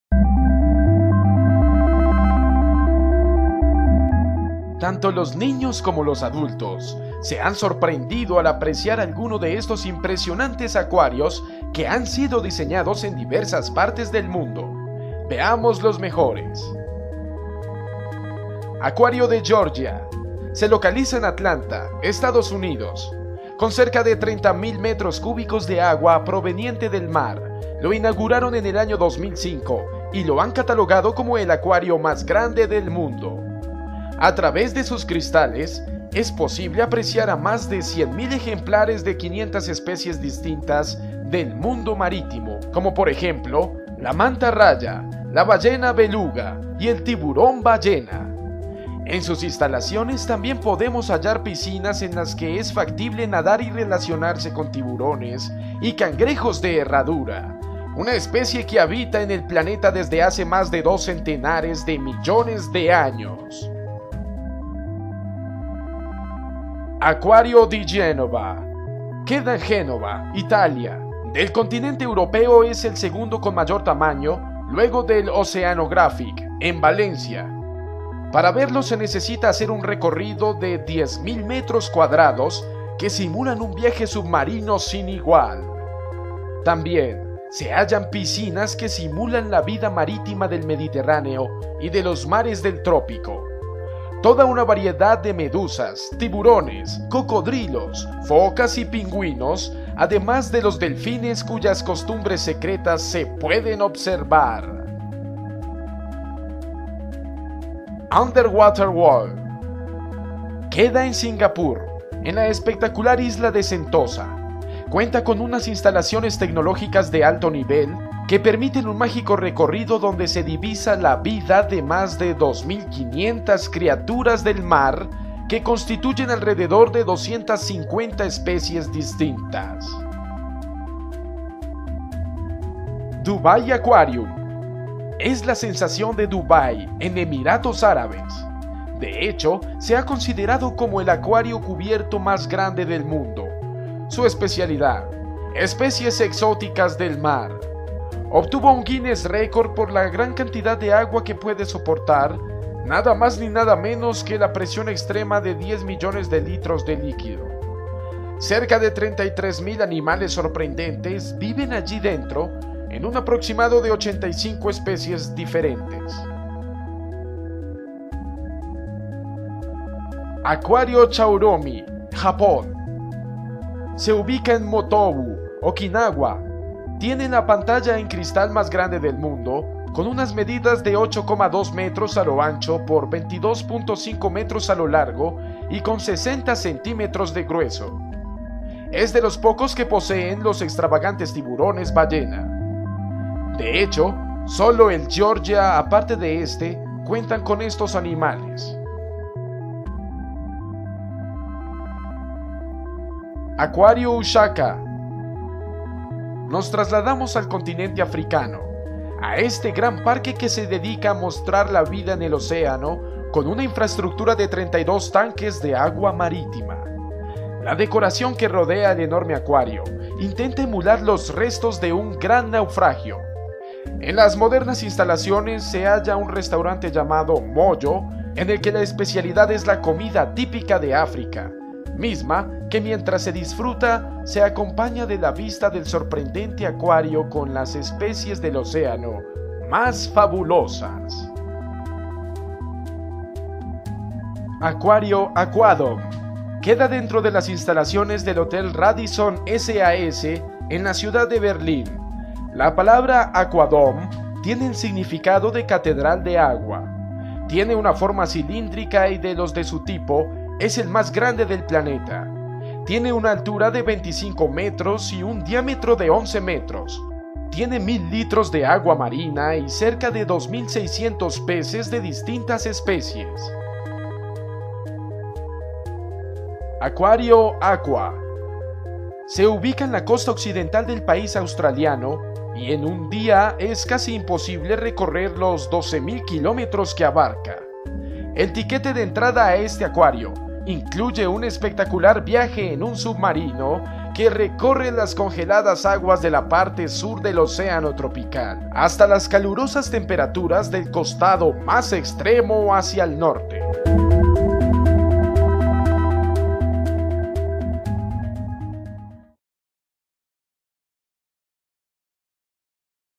Los acuarios más hermosos del mundo
Prepárate para sumergirte en un viaje visual a través de los acuarios más impresionantes y cautivadores que existen en el planeta. Desde el Acuario de Georgia en Estados Unidos hasta el Acuario Di Genova en Italia, este video te llevará a explorar maravillas subacuáticas que no solo albergan una increíble variedad de criaturas marinas, desde peces diminutos hasta majestuosas ballenas y tiburones, sino que también atraen a turistas de todas partes del mundo.

Adéntrate en la belleza de estos espacios subacuáticos únicos, descubriendo la magia de sus diseños arquitectónicos y la diversidad de vida marina que albergan. Acompáñanos para maravillarte con la grandeza y la hermosura de los acuarios más destacados del mundo.